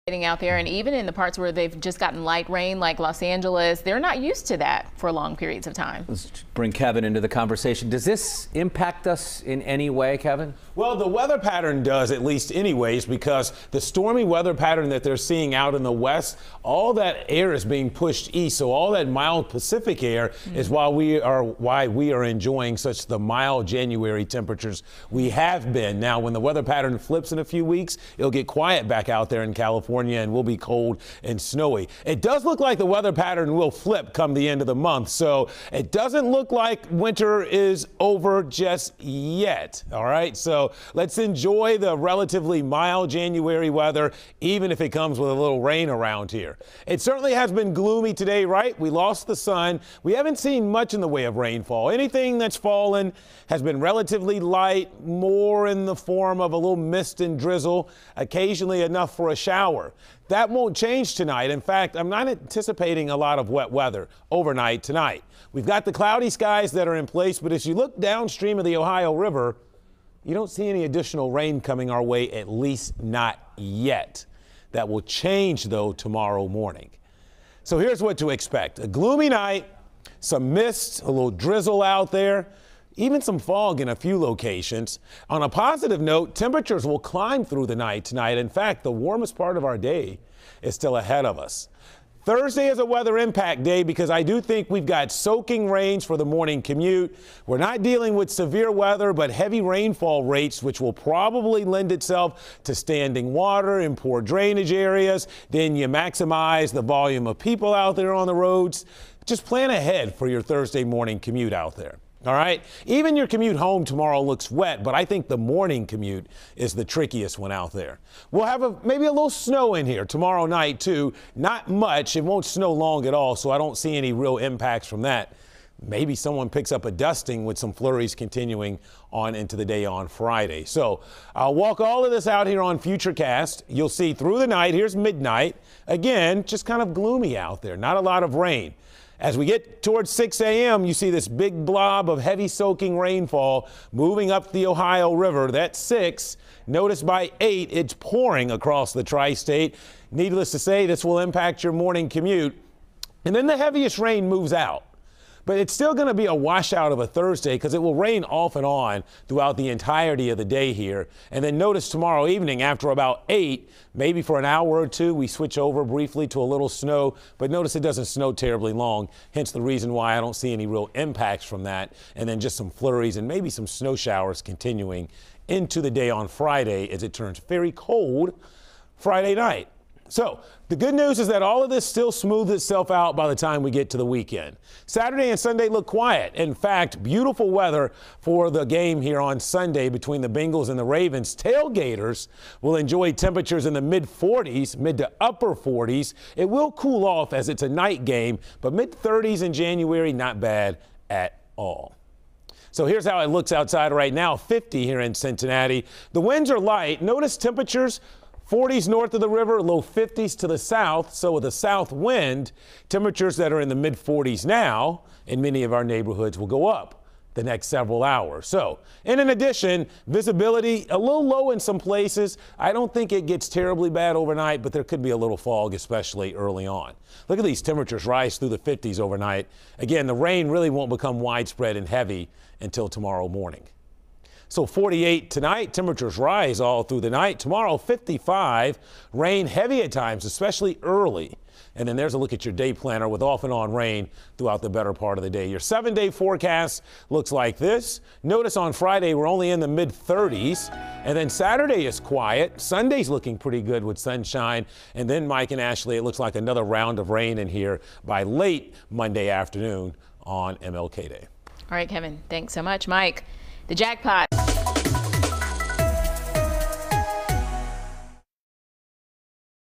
A Soggy Thursday
Rain will have a significant impact on the Thursday morning commute.